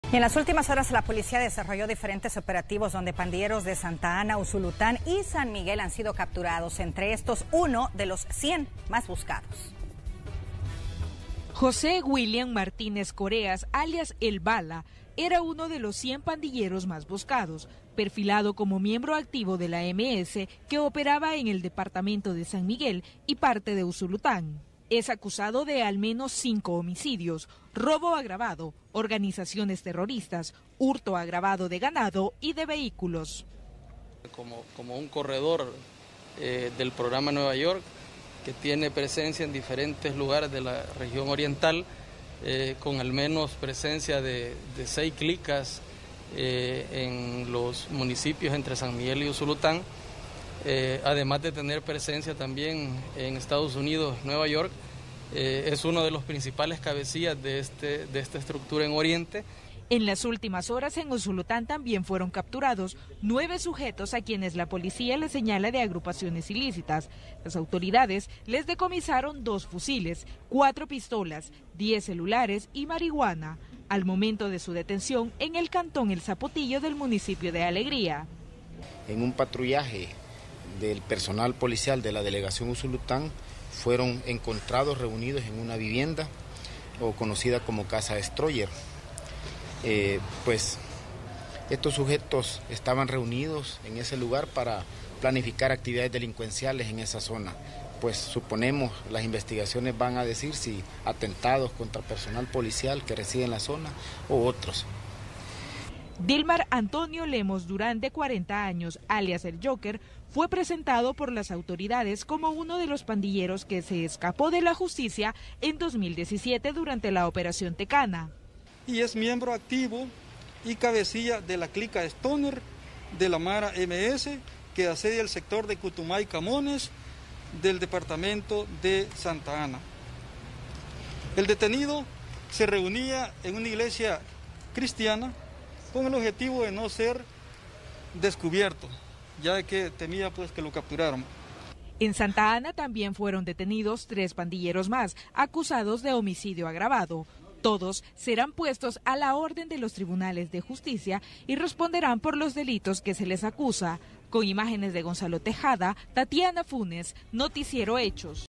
Cae uno de los 100 pandileros más buscados
José William Martínez Coreas alias el balas es miembro activo de la MS que operaba en San Miguel y parte de Usulután. Fue capturado en los últimos operativos realizados por la PNC donde atraparon a otros 12 delincuentes.